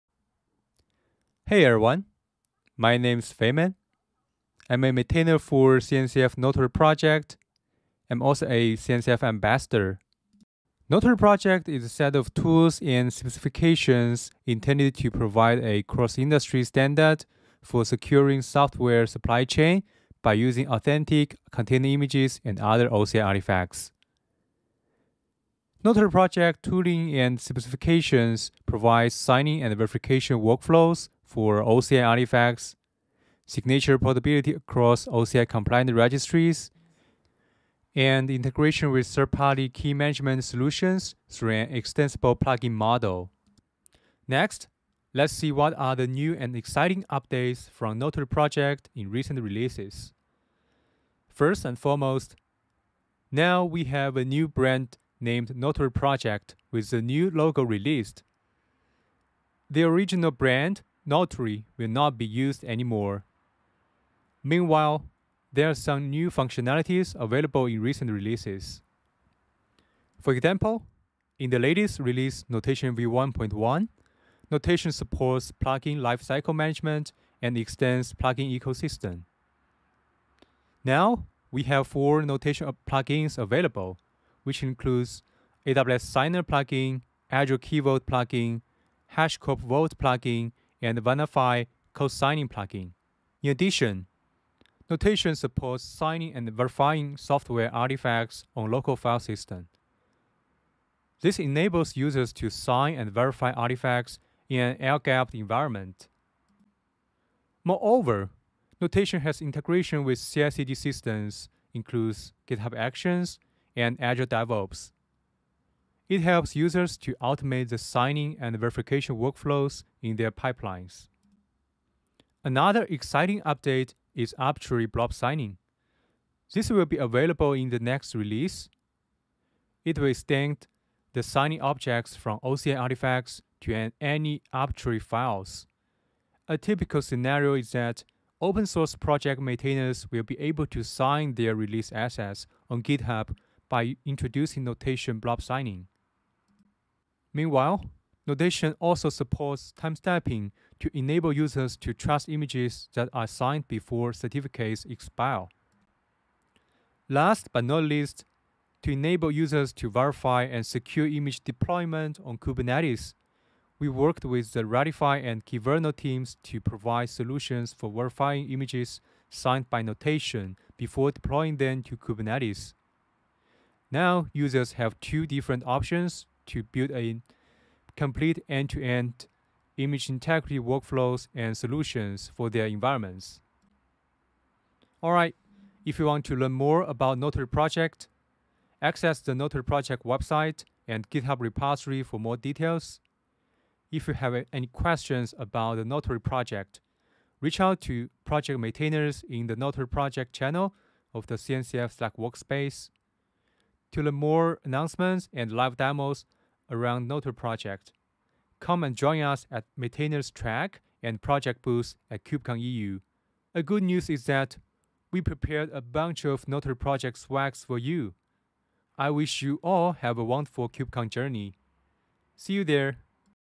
Join Notary Project at KubeCon and CloudNativeCon Europe 2024!
Notary Project is a CNCF Incubating project. We are pleased to share some exciting updates and live demos of Notary Project with its ecosystem partners at KubeCon + CloudNativeCon Europe and host a project booth for interactive communication in Paris.

Want to have the first glance at the exciting updates of Notary Project in 2024? This video by Feynman Zhou, a Notary Project maintainer and CNCF Ambassador, gives you a pre-event preview.